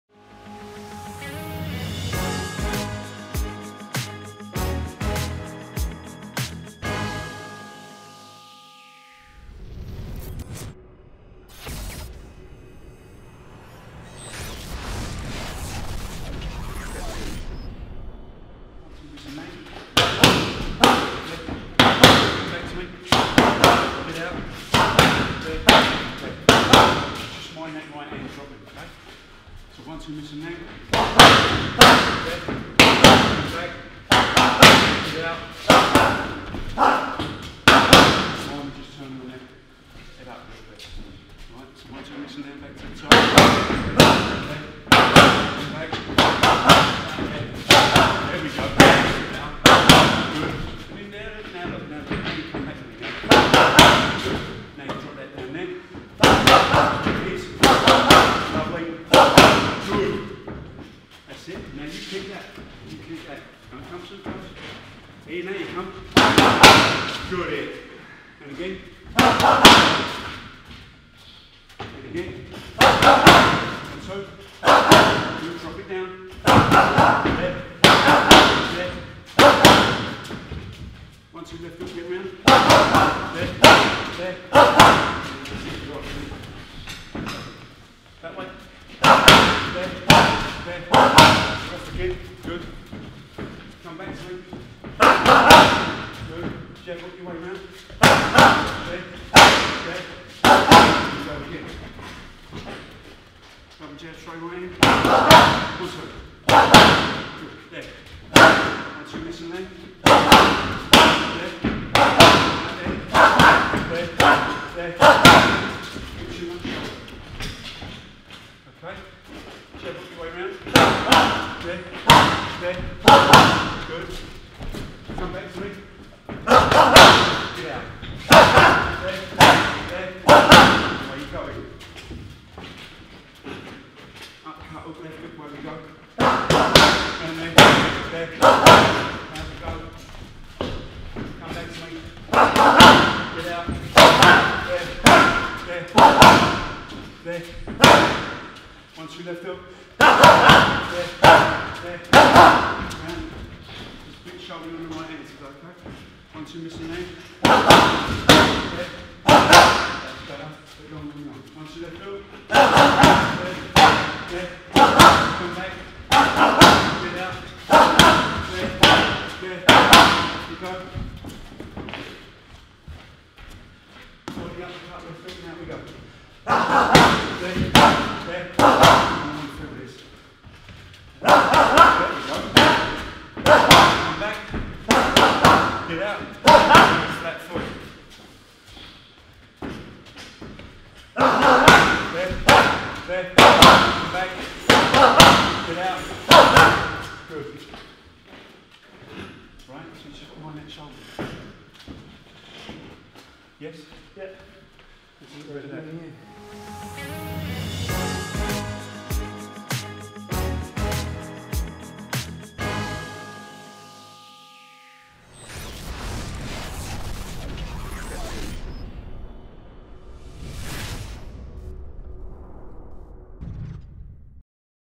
THE MILCH MAN! UNDEFEATED TONY MILCH (11-0) SMASHES THE PADS WITH IAN BURBEDGE
THE MILCH MAN! UNDEFEATED TONY MILCH (11-0) SMASHES THE PADS WITH TRAINER IAN BURBEDGE